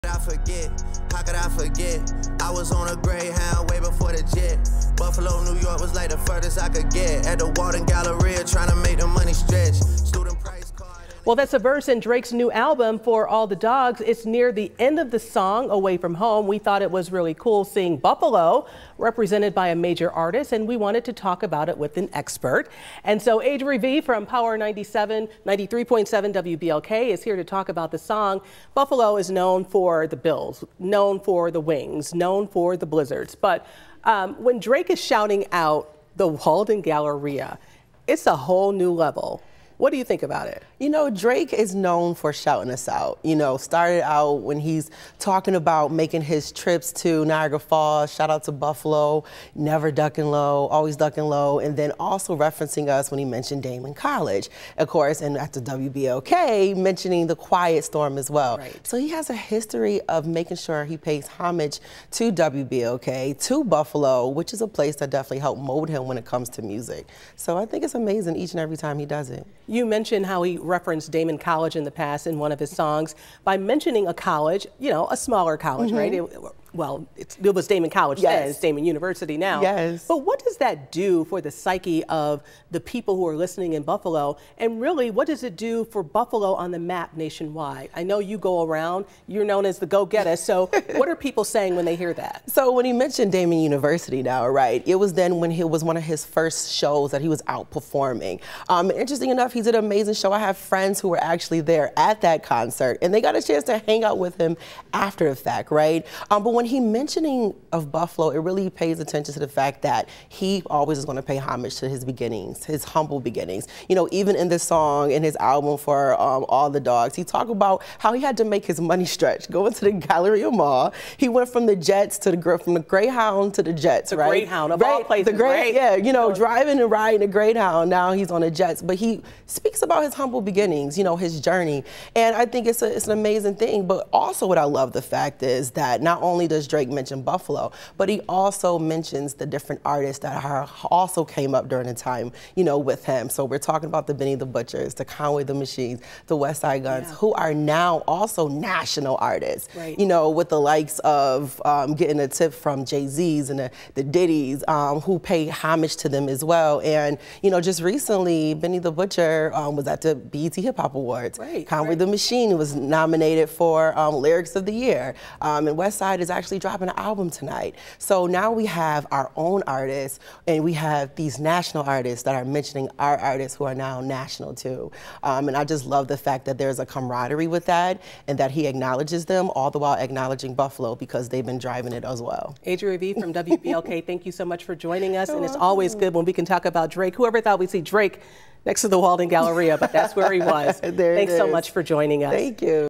Drake reminisces about Buffalo, the Walden Galleria on his latest album
2 On Your Side's Claudine Ewing talked with ADRI V from Power 93.7 WBLK about Drake and his fondness for referencing Western New York in his songs.